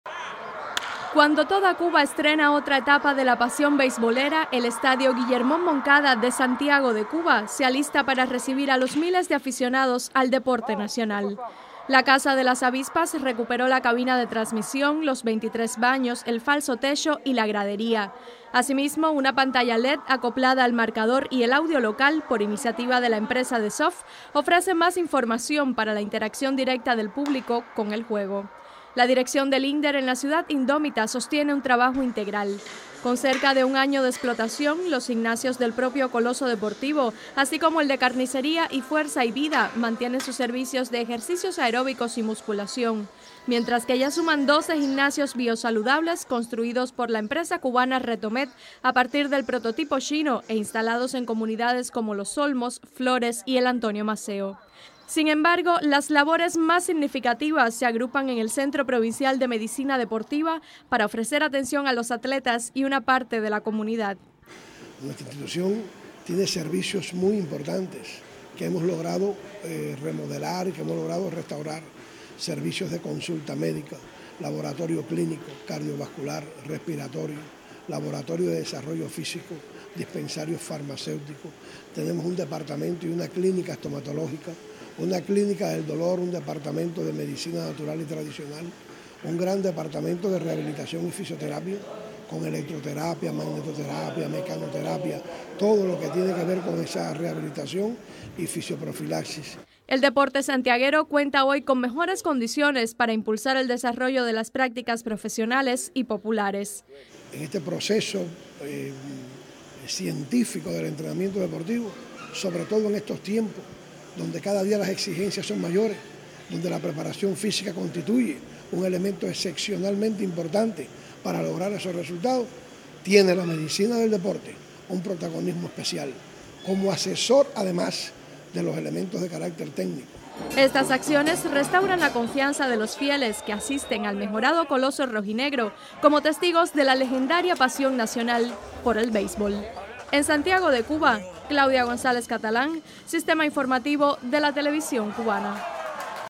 Instalaciones deportivas santiagueras impulsan el desarrollo deportivo.
La Dirección Provincial del INDER en Santiago de Cuba alista sus instalaciones para impulsar el desarrollo del deporte profesional y popular. Más detalles en el siguiente reportaje